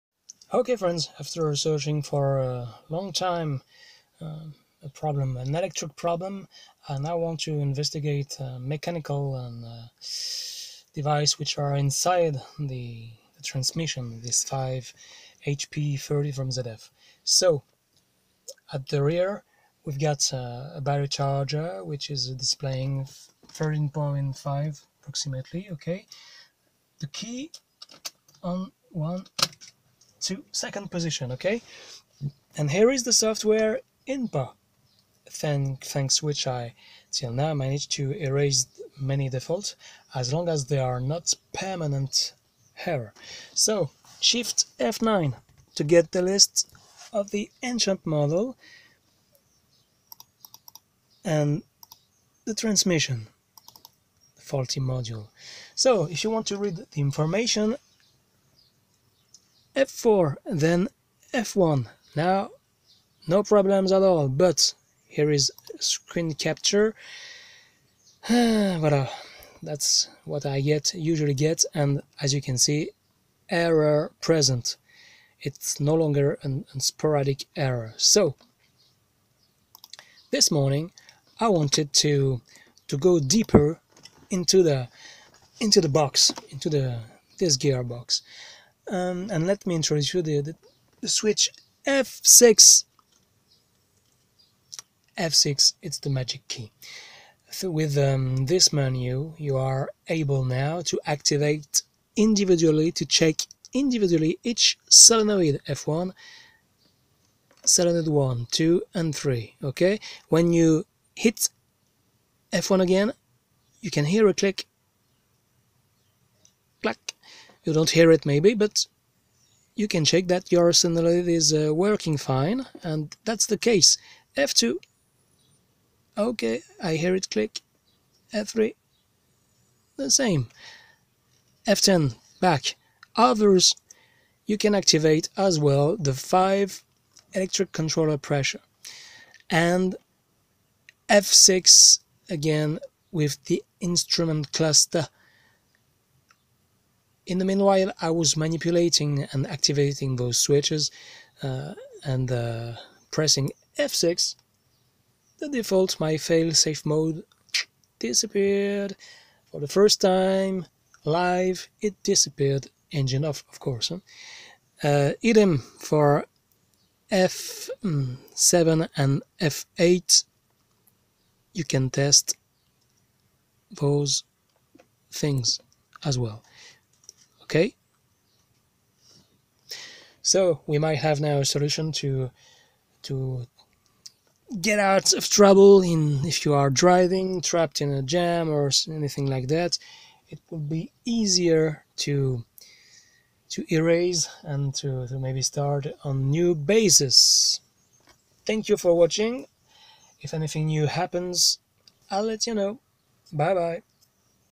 Transmission failsafe program BMW E 38 part 4/5: Solved! Warming up solenoïds with INPA
Limp mode, programme sécurité boîte de vitesse, 5hp30 ZF failsafe program, mise en sécurité bva, tranny fail safe program BMW 740i